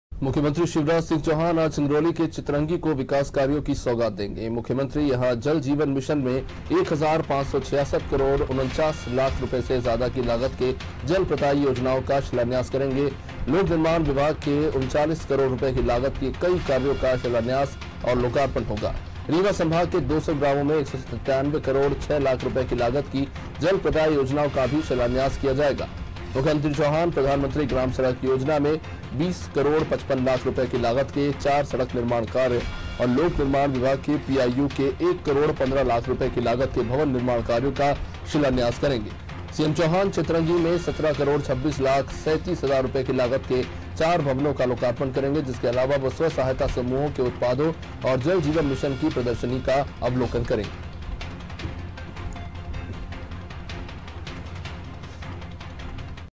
MP NEWS : सीएम शिवराज सिंगरौली को देंगे सौगात |Aaj ki Taza Khabar | Anaadi Tv |Hindi News
मुख्यमंत्री शिवराज सिंह चौहान आज सिंगरौली के चितरंगी को विकास कार्यों की सौगात देंगे... मुख्यमंत्री यहां जल-जीवन मिशन में 1 हजार 566 करोड़ 49 लाख रुपये से ज्यादा लागत की जल-प्रदाय योजनाओं का शिलान्यास करेंगे... लोक निर्माण विभाग के 39 करोड़ रुपये की लागत के कई कार्यों का शिलान्यास और लोकार्पण होगा... रीवा संभाग के 200 ग्रामों में 197 करोड़ 6 लाख रूपये लागत की जल-प्रदाय योजनाओं का भी शिलान्यास किया जायेगा... मुख्यमंत्री चौहान प्रधानमंत्री ग्राम सड़क योजना में 20 करोड़ 55 लाख रुपये लागत के चार सड़क निर्माण कार्य और लोक निर्माण विभाग पीआईयू के एक करोड़ 15 लाख रुपये लागत के भवन निर्माण कार्यों का शिलान्यास करेंगे... सीएम चौहान चितरंगी में 17 करोड़ 26 लाख 37 हजार रुपये लागत के चार भवनों का लोकार्पण करेंगे... इसके अलावा वो स्व-सहायता समूहों के उत्पादों और जल-जीवन मिशन की प्रदर्शनी का अवलोकन भी करेंगे...